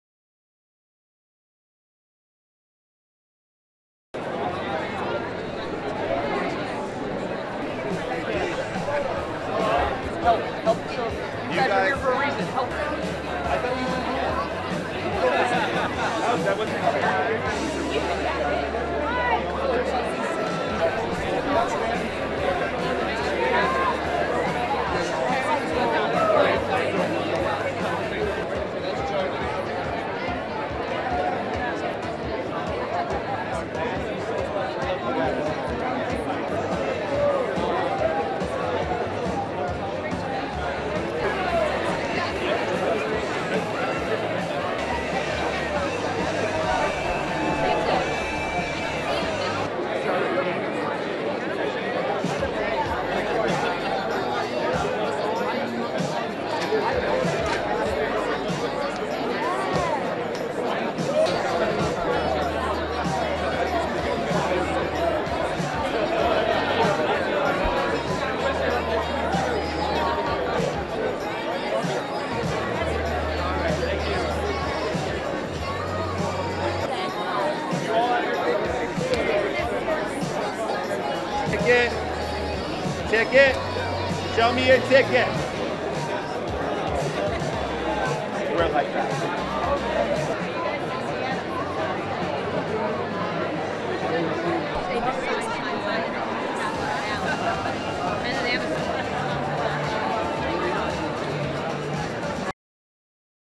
THE DEFENDERS - COMIC CON - SAN DIEGO - AUTOGRAFOS - FANS
El elenco de la serie de Marvel "THE DEFENDERS", asistieron al Comic Con de San Diego, para presentar la Serie, donde dieron autografos a todos sus fans.

Elenco: Charlie Cox, Krysten Ritter, Mike Colter y Finn Jones.

Estreno en Netflix a partir del 18 de Agosto.